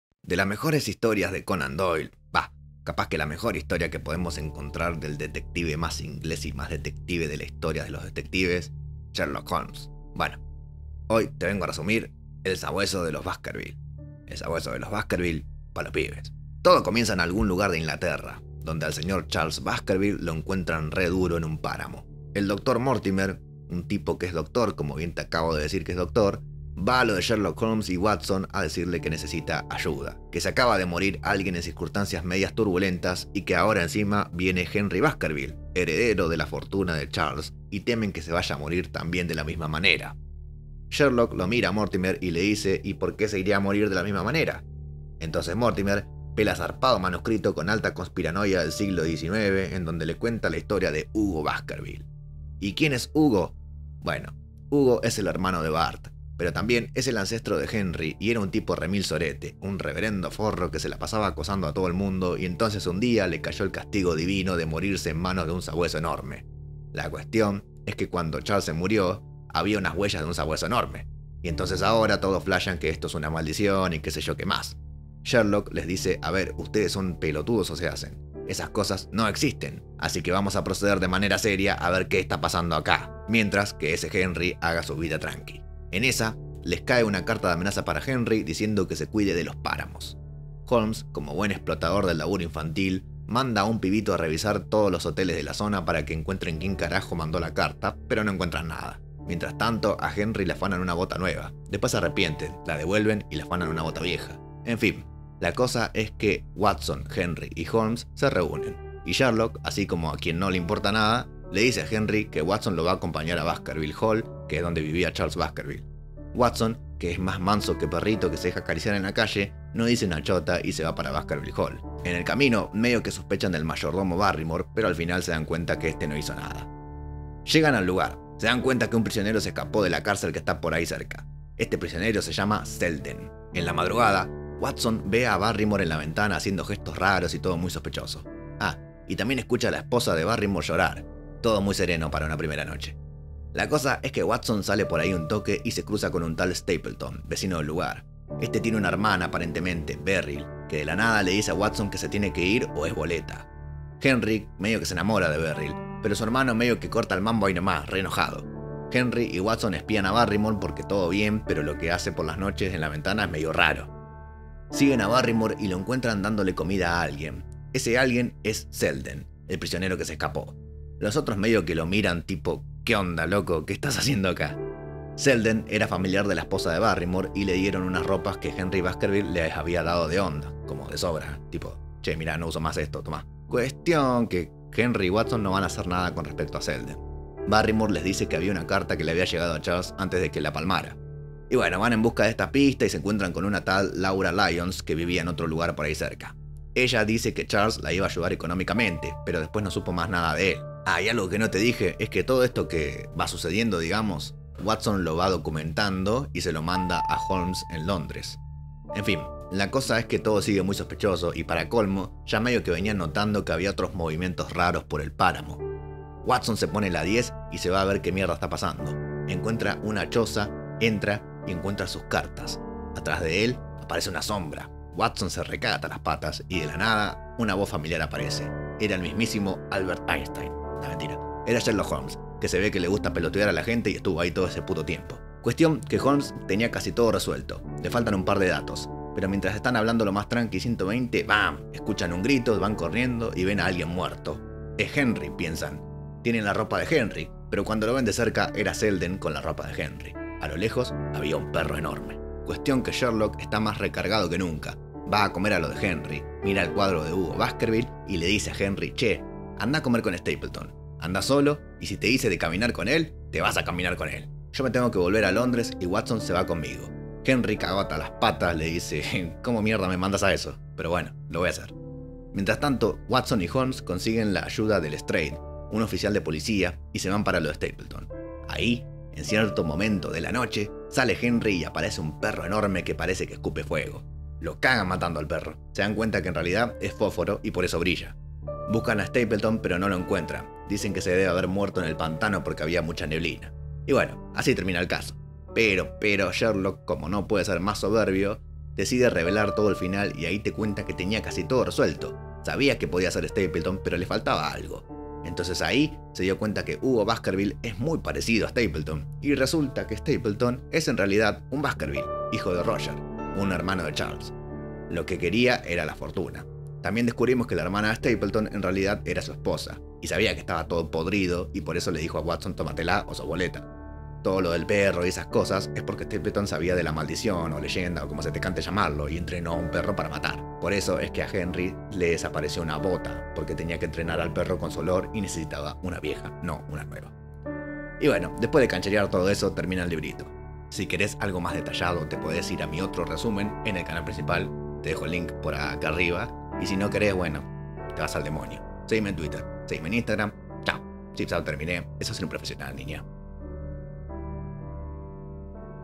EL SABUESO DE LOS BASKERVILLE | RESUMEN COMPLETO | ARTHUR CONAN DOYLE | LIBROS PA' LOS PIBES
Y acá va un resumen de El sabueso de los Baskerville, zarpada novela de Arthur Conan Doyle, con el detective más detectivezco de todos: Sherlock

Ah, y por si te interesa una versión un poco más seria para estudiar o para lo que sea, pasate por mi otro canal, que ahí tenes muchas más cosas: